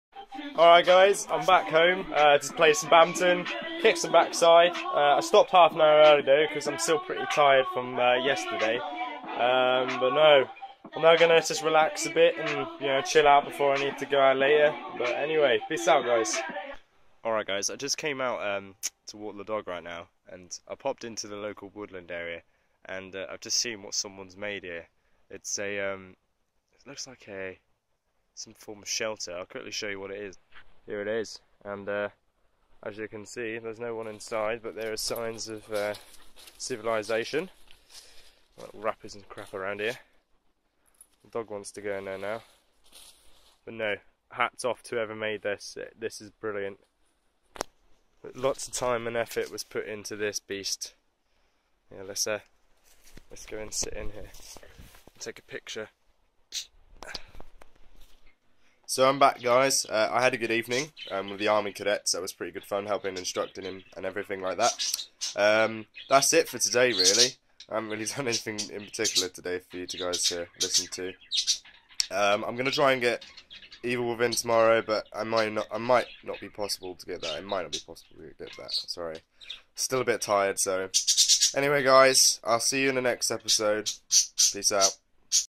Vlog 02 - Motivation?
In this second episode of my vlog I just talk about the few things that have been happening that day and what I am up to in the following day!

Peace out.